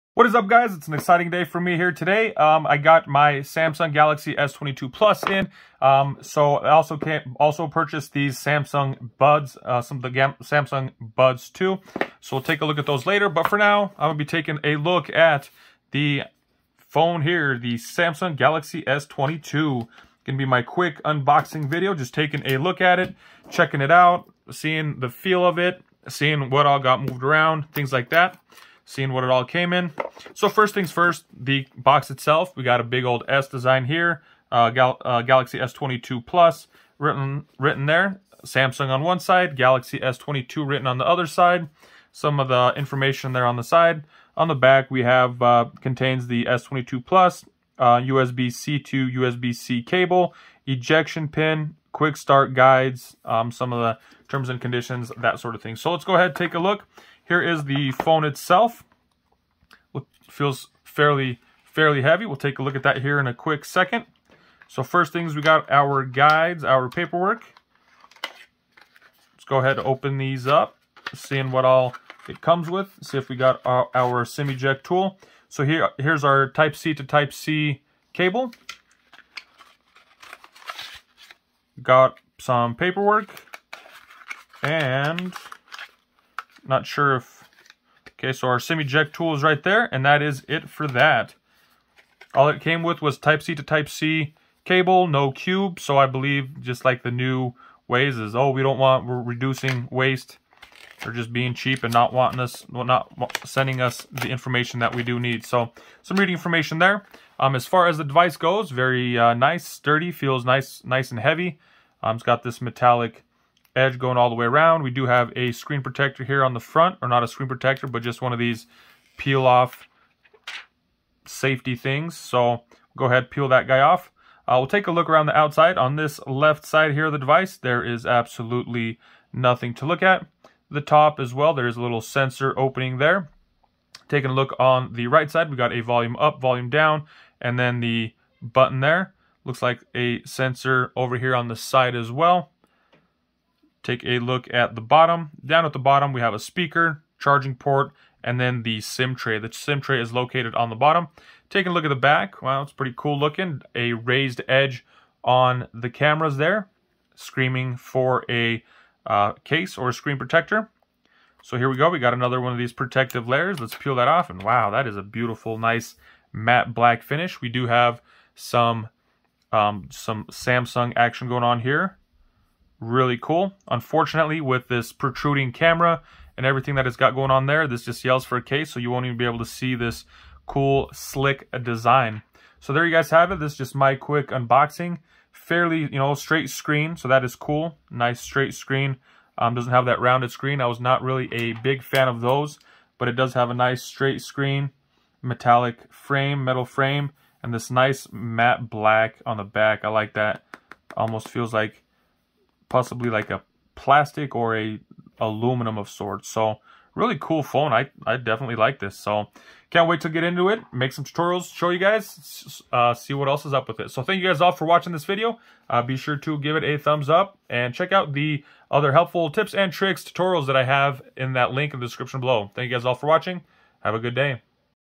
Samsung galaxy S22 plus unboxing
Samsung Galaxy s22 plus unboxing. first look at the Samsung Galaxy s22 plus.